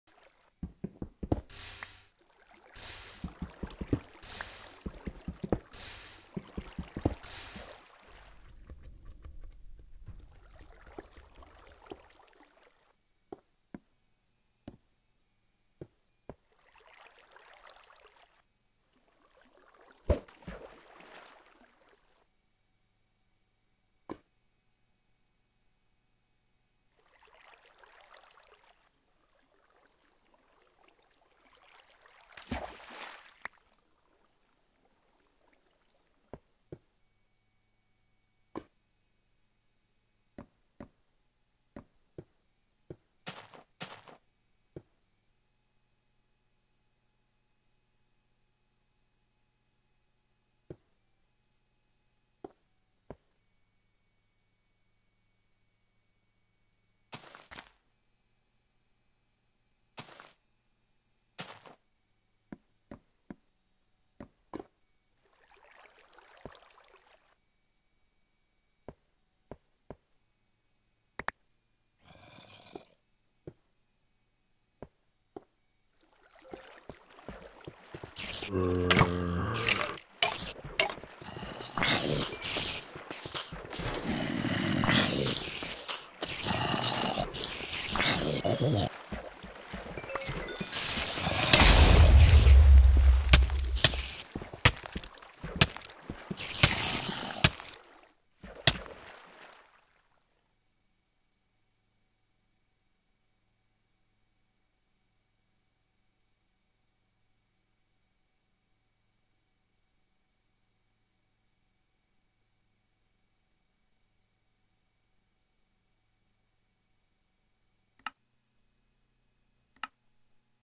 SkyGrid death again.avi
This is a quick look at SkyGrid, I have died many times to this point now see what happens.  If you try this map drop me a line as to how you do.